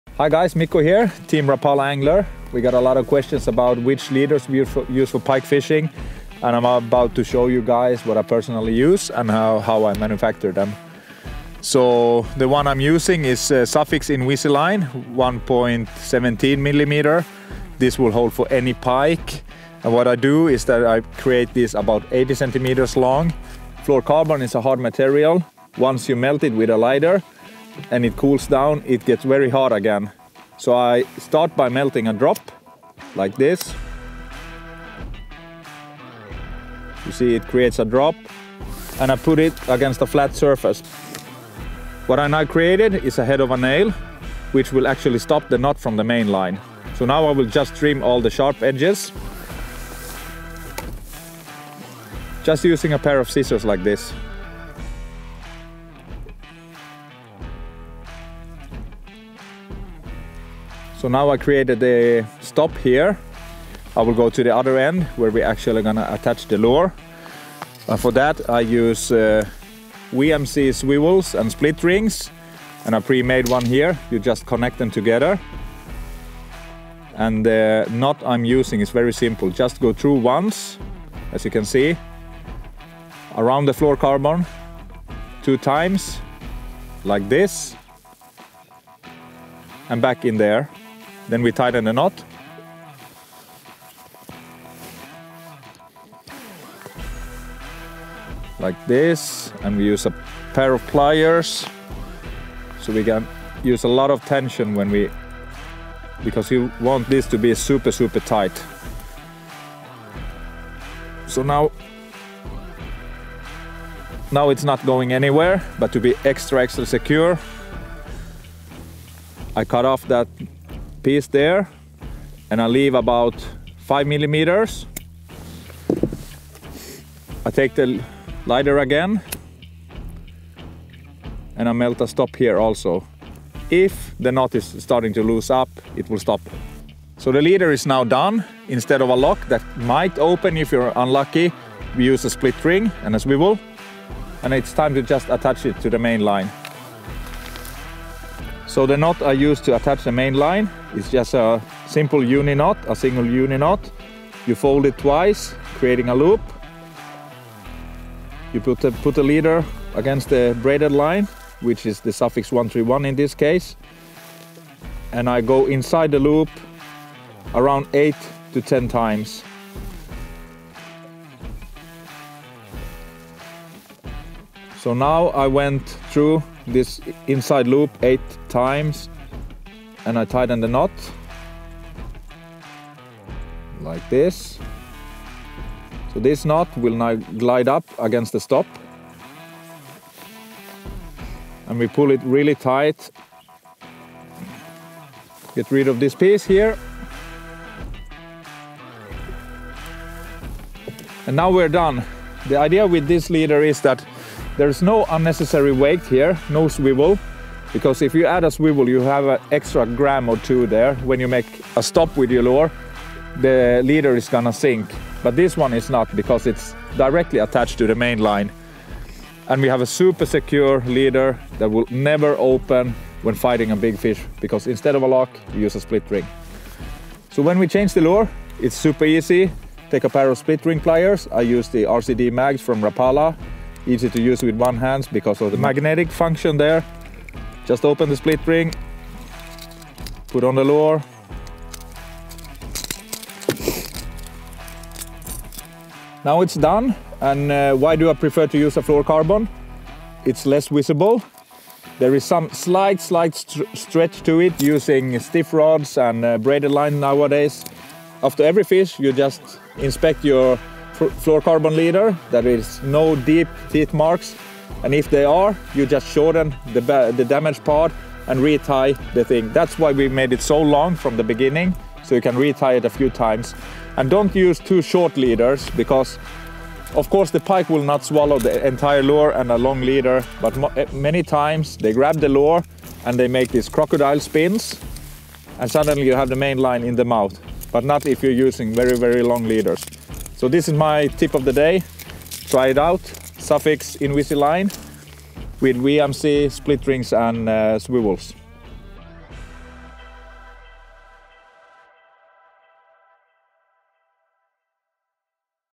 HOW-TO | Tying A Fluorocarbon Pike Leader With Mikko Seppänen | Sufix®
Swedish Pike Fight champion, and enthusiastic predator fisherman, Mikko Seppänen, shares his tried and tested big-pike leader with you in this video.

Swapping out titanium wire leaders for thicker fluorocarbon leaders when fishing for big toothy fish like the northern pike may seem unusual to some, but Mikko and many other anglers have discovered that fluorocarbon can be more reliable due to its predictability, among other benefits.

Mikko's leader, whilst fairly unconventional, has proven to be very successful for him in high pressure situations such as tournaments.

What do you think about this leader? Will you try it?